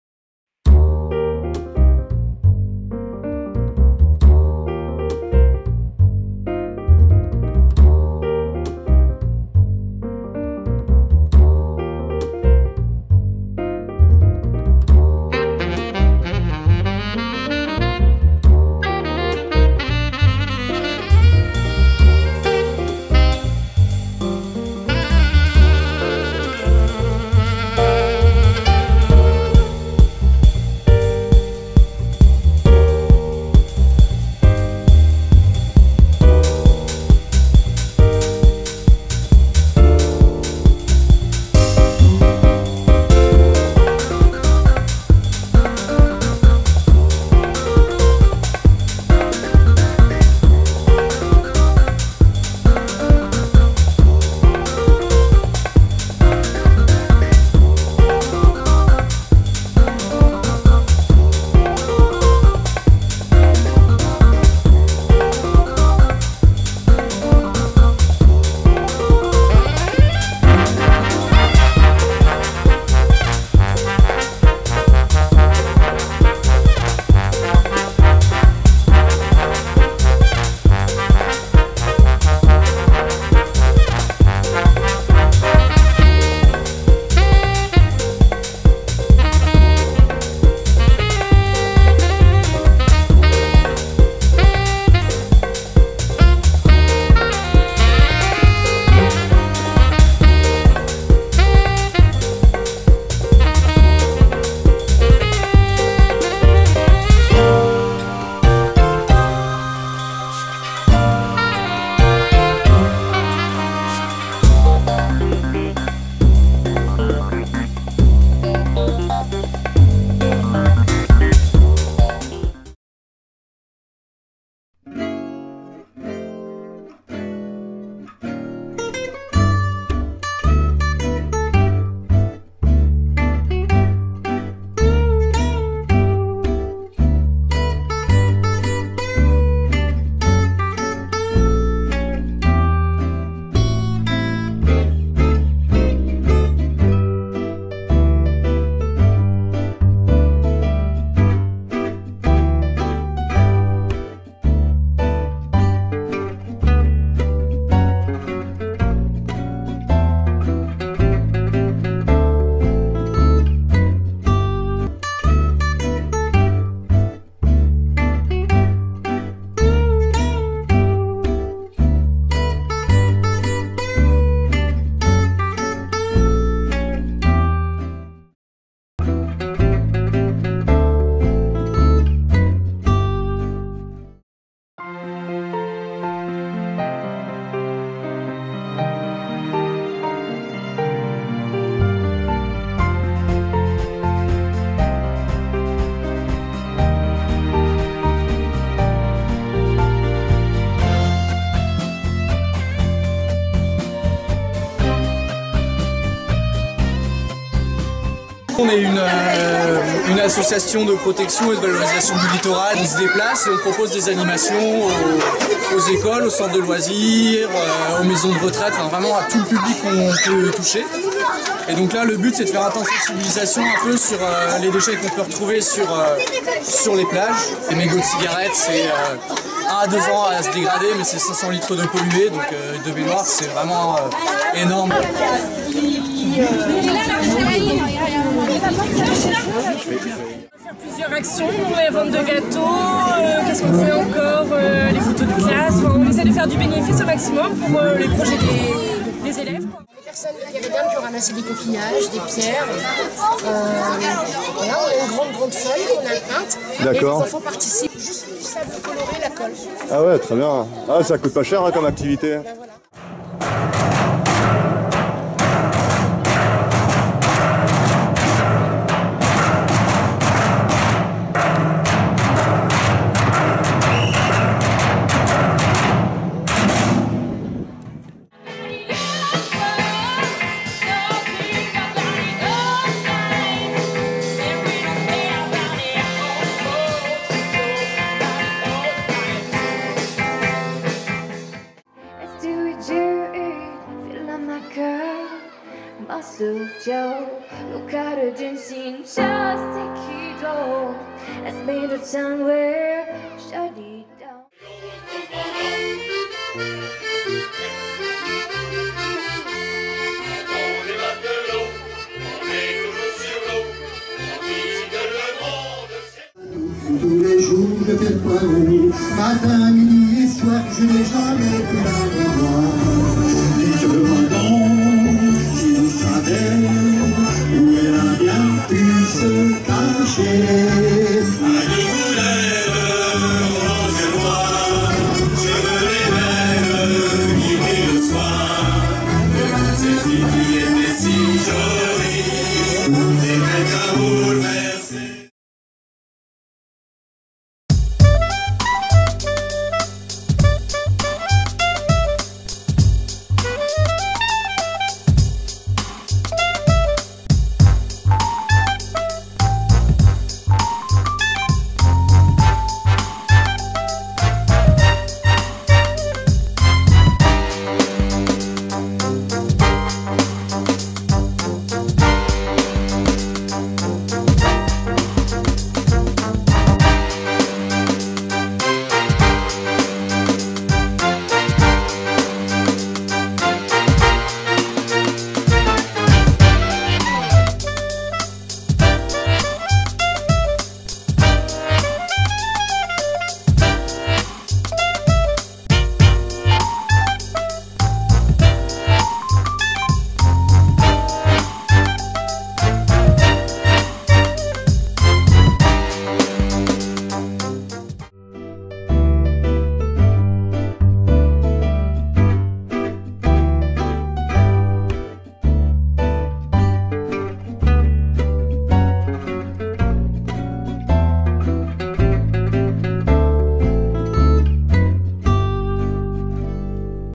Festivités 2014 (avchd mp4 HD 720 60)
Fête de quartier organisée par ses habitants bénévoles et partenaires.Kermesse, ateliers scientifiques, de bricolage(ptits déb).,maquillage,asso surfrider, ape paul dukas. Concerts: romane, ysabel rose, sweet monsters,les tanbours de kérinou, vents d'iroise stool. Moules frites.